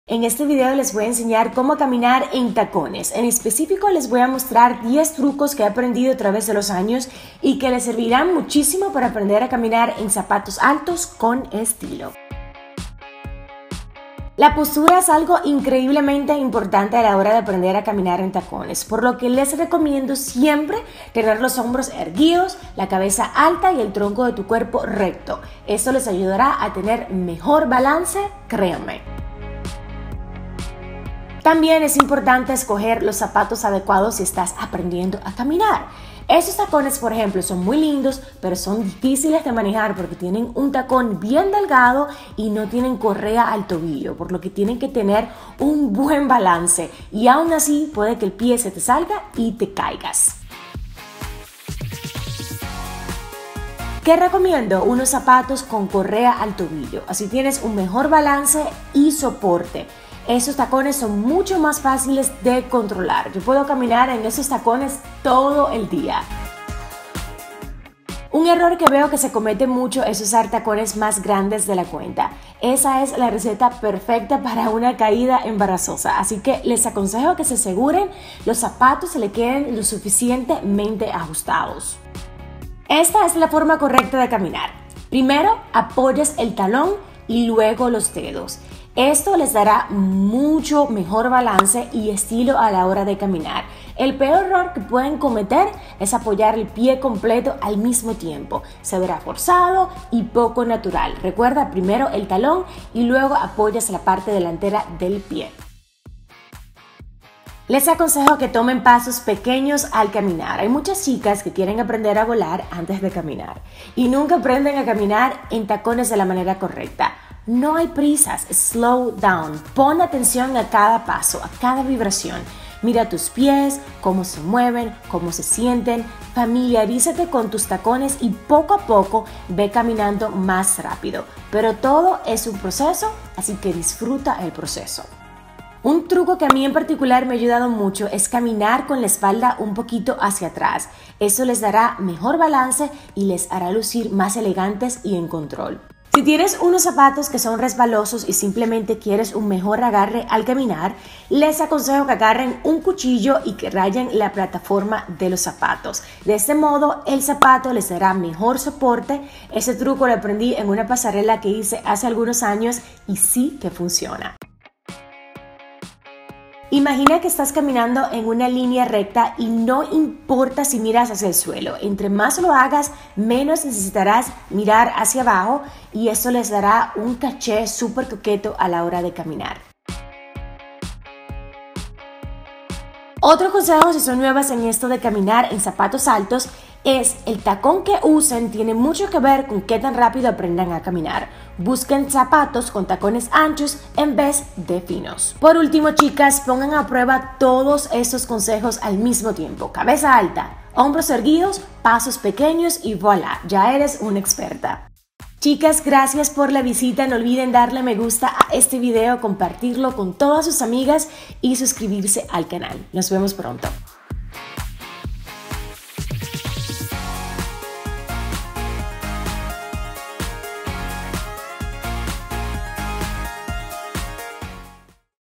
10 TRUCOS PARA APRENDER A CAMINAR EN TACONES ALTOS | Doralys Britto
Holas chicas! En este video les voy a revelar 10 trucos fantásticos para aprender a caminar en tacones. Son trucos que he aprendido con los años y que me han servido muchísimo, no solo a caminar en tacones, sino a caminar con estilo!

Si te gusta el video compártelo me ayudarías muchísimo!!
Comenta+Comparte+Like